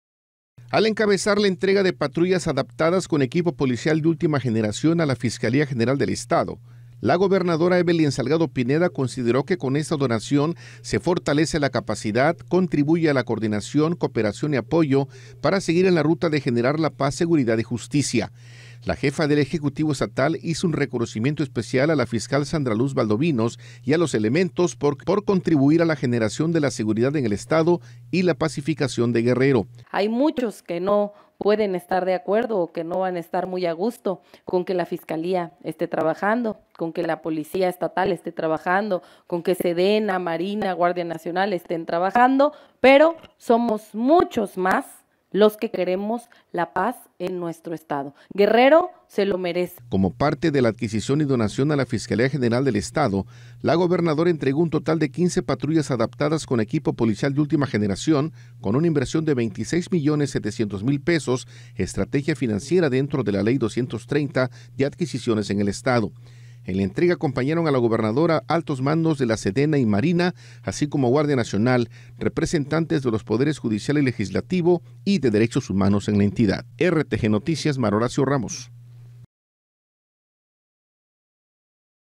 RTGnoticias | Entrega Guerrero en donación 15 patrullas equipadas a la FGE - 30 de agosto del 2023
Al encabezar la entrega de patrullas adaptadas con equipo policial de última generación a la Fiscalía general del estado, la gobernadora Evelyn salgado Pineda, consideró que con esta donación se fortalece la capacidad, contribuye a la coordinación, cooperación y apoyo para seguir en la ruta de generar la paz, seguridad y justicia.